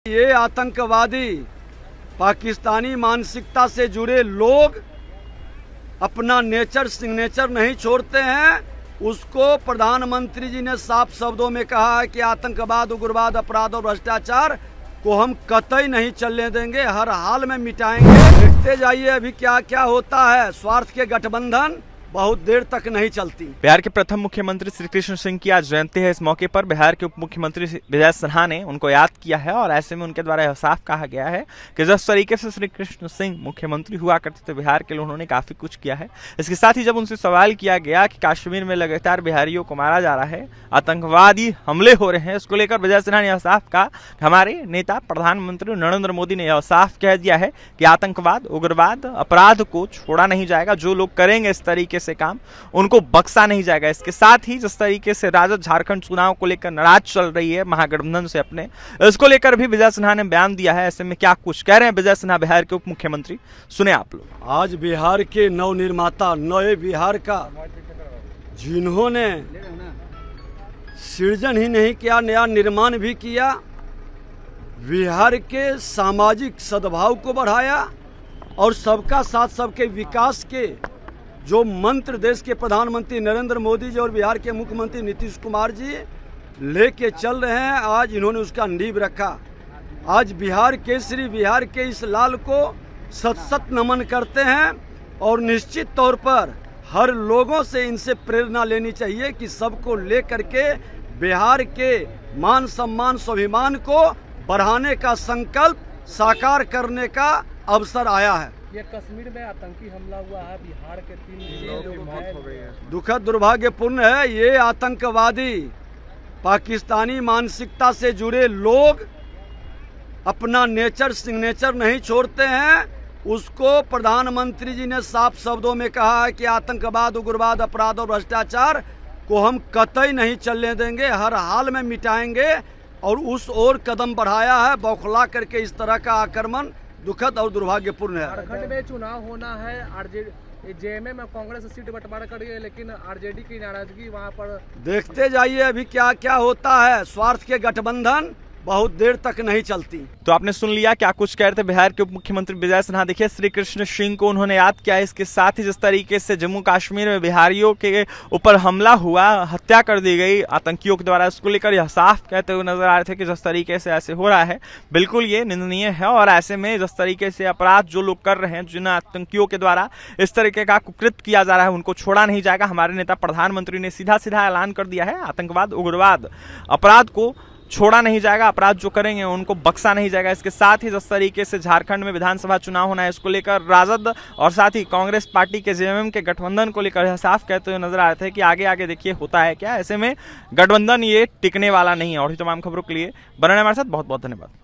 ये आतंकवादी पाकिस्तानी मानसिकता से जुड़े लोगों को छोड़ा नहीं जाएगा उपमुख्यमंत्री Vijay Sinha की..
ये आतंकवादी पाकिस्तानी मानसिकता से जुड़े लोगों को छोड़ा नहीं जाएगा उपमुख्यमंत्री Vijay Sinha की खुलेआम चेतावनी !